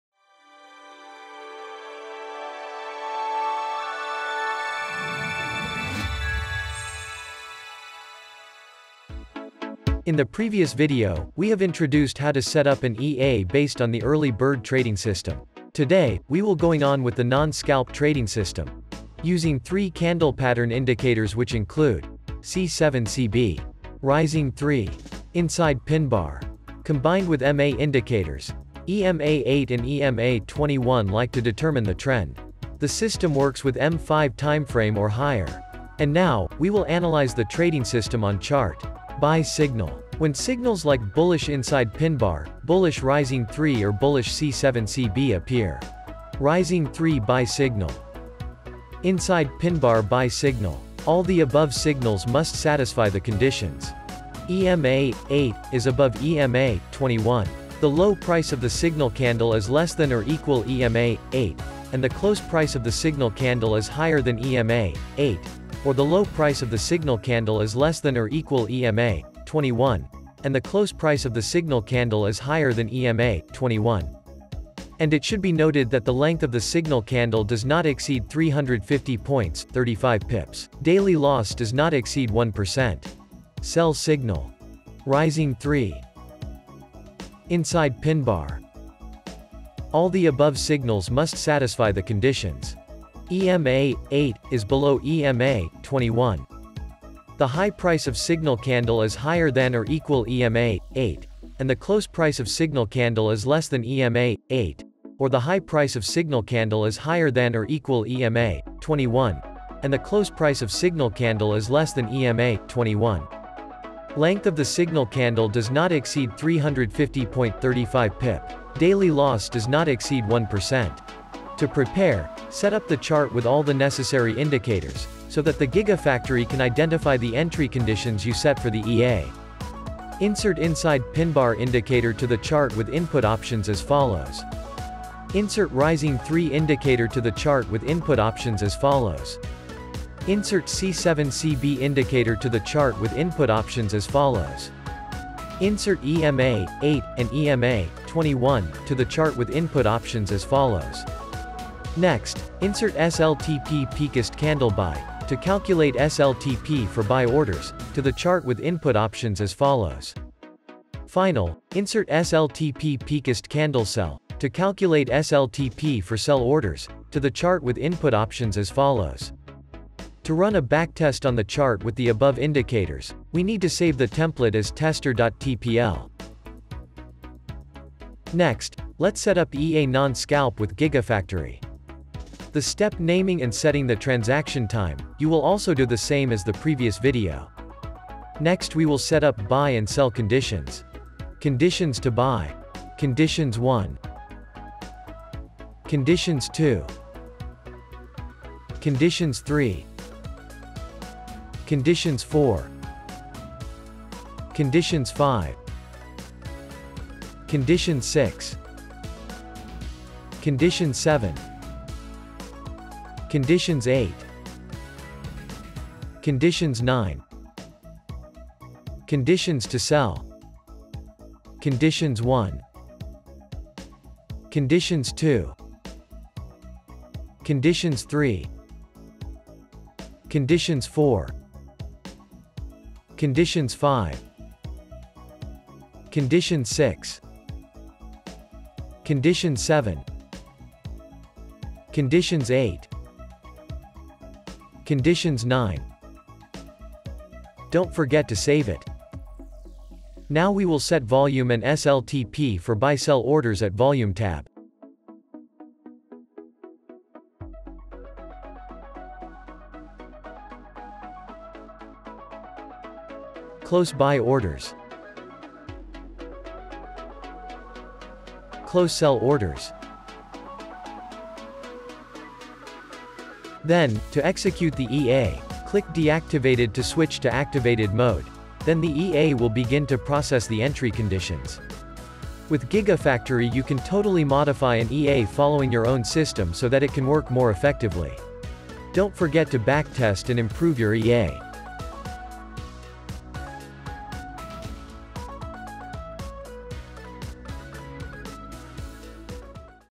NONSCALP SYSTEM I Tutorial set up an EA GIGAFACTORY
In the previous video, we have introduced how to set up an EA based on the Early Bird trading system. In this video, we will going on with the NonScalp trading system.The system is scalping trading, safe and effective.

Community: @FxceSTP_VN
Technical support: @FXCEEA
FXCE - The New Era of Social Trading